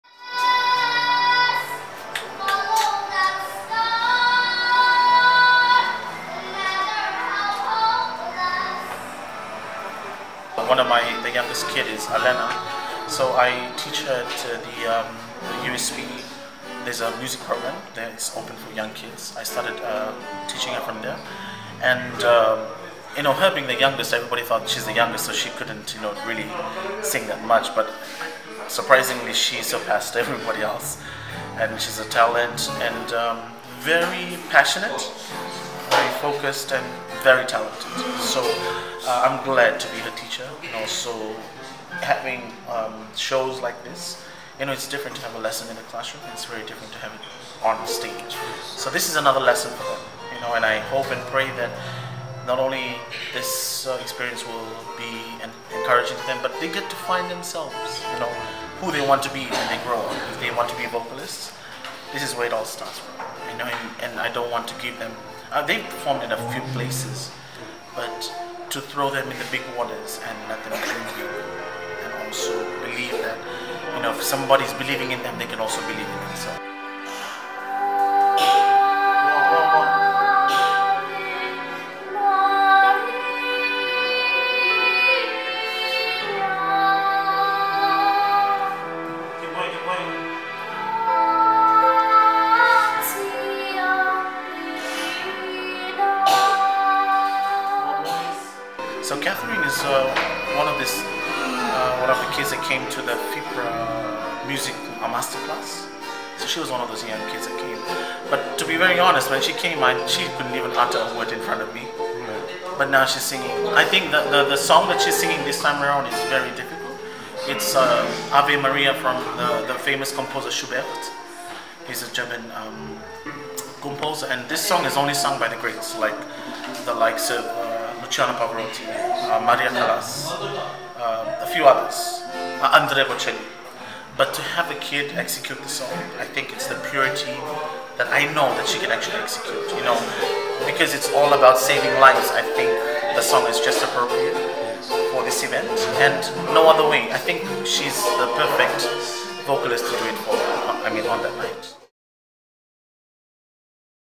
LOCAL MUSIC | Seven-year-old Alena ready to perform
ALENA Vudikaria, Fiji’s seven-year-old singing sensation will make her big debut at the I Believe fundraiser for the St John’s Association gala event this Saturday at the Grand Pacific Hotel.

The youngster will showcase her amazing vocal talent alongside a star-studded cast including Pasifika Prima Voce – which features the amazing vocal talent of Sunia Soko Loga, and multi FPRA Music Award winners Yanu Boyz.